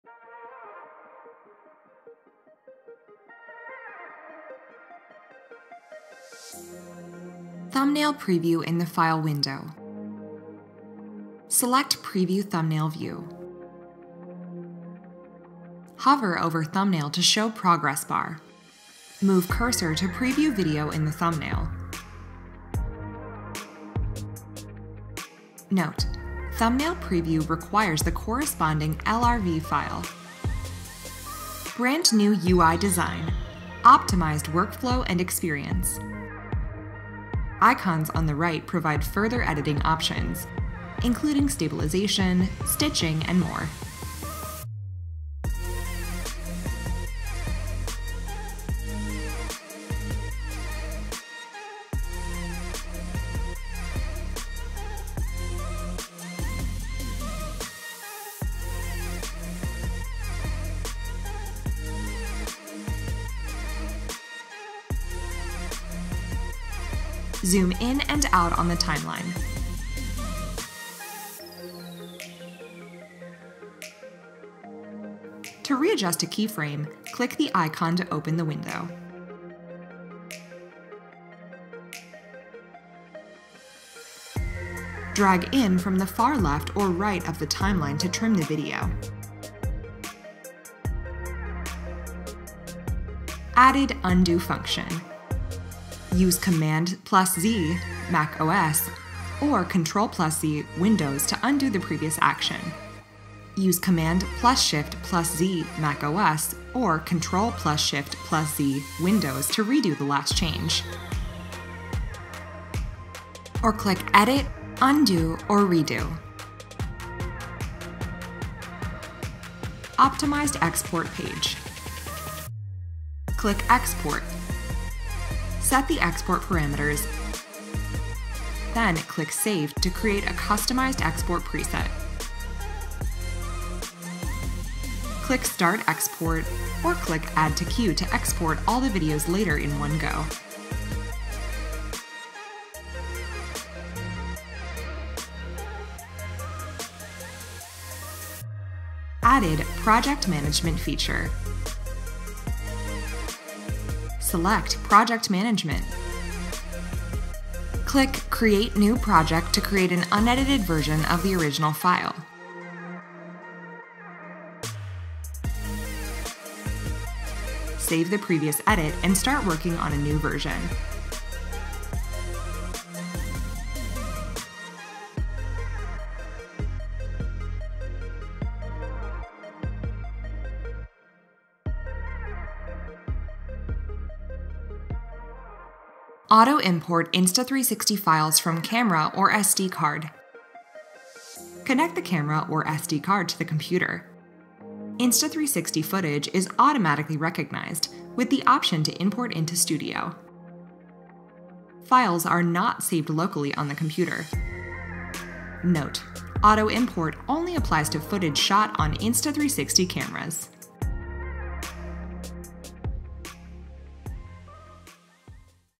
Insta360 Studio Update For Mac and Windows
Desktop editing just got A LOT easier with Insta360 Studio! The free update for Mac and Windows includes:
- Completely redesigned user interface
- New Project Management panel to edit multiple versions of the same files
- New thumbnail preview option
- Auto importing
- Customizable export presets
- Batch exporting
- M1 Mac compatibility
- And more!

00:07 "Thumbnail preview"in the file window
00:31 Band New UI design
01:32 Added"Undo"function
02:00 Optimized export page
02:29 Added "Project Management"feature
03:05 Auto import Insta360 files